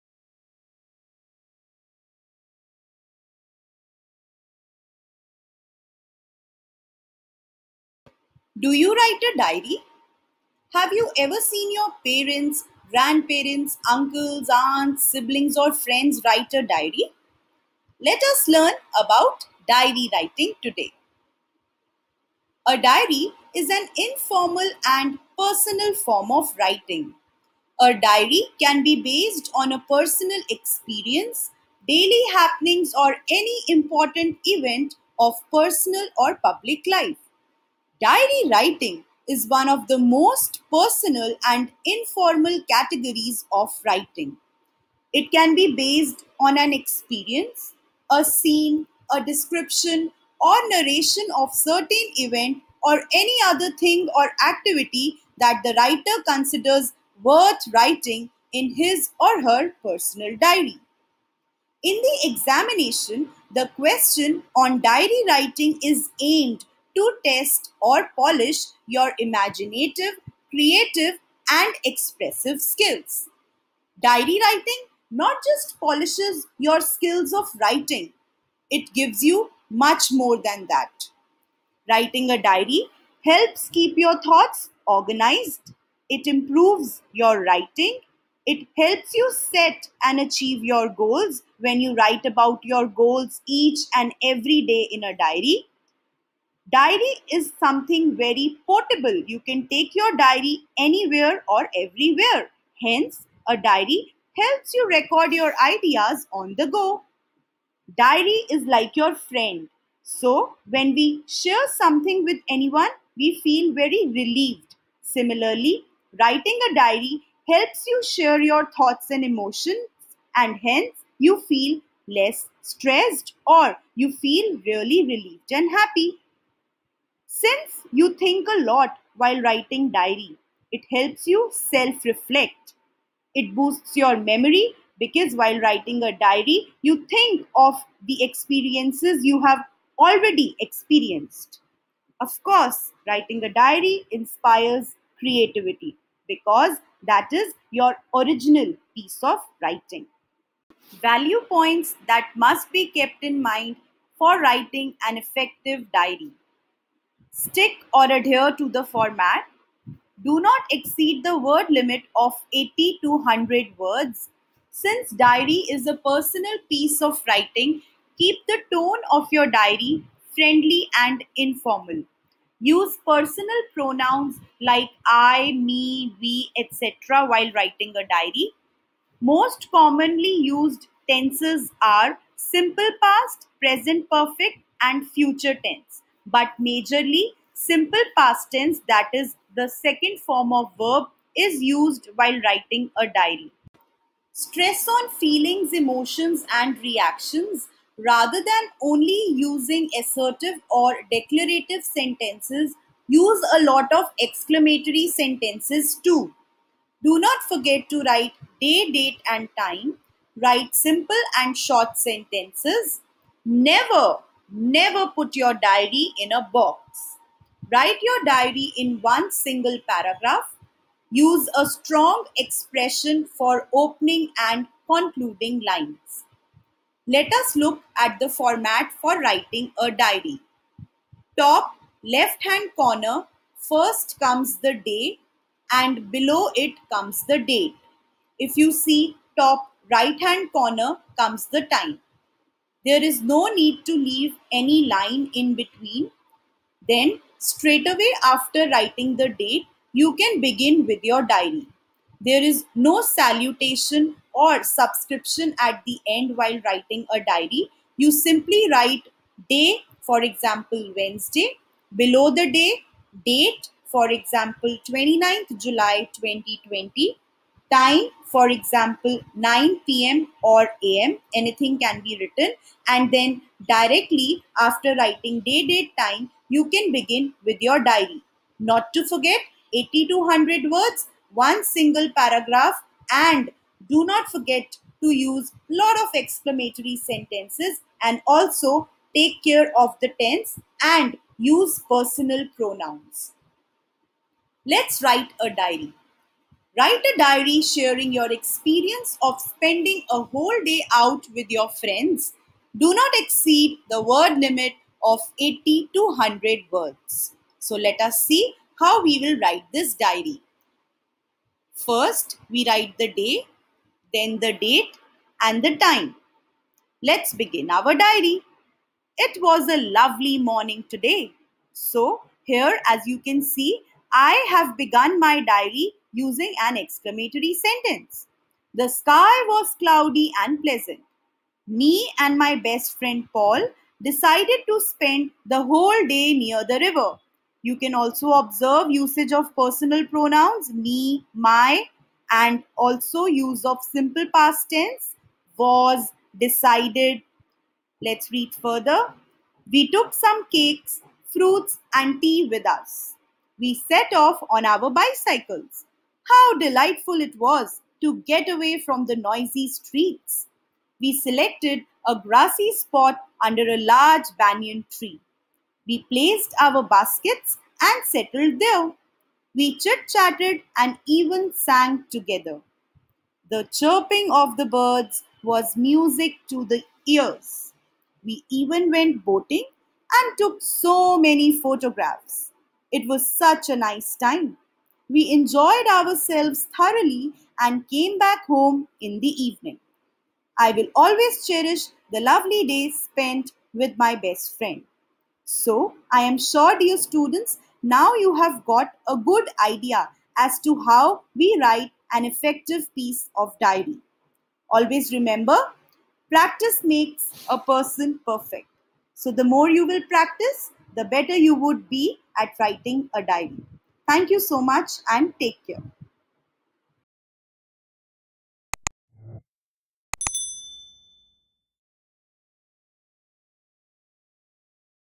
|| Complete explanation: DIARY WRITING ||
Let us learn all about writing an effective diary:)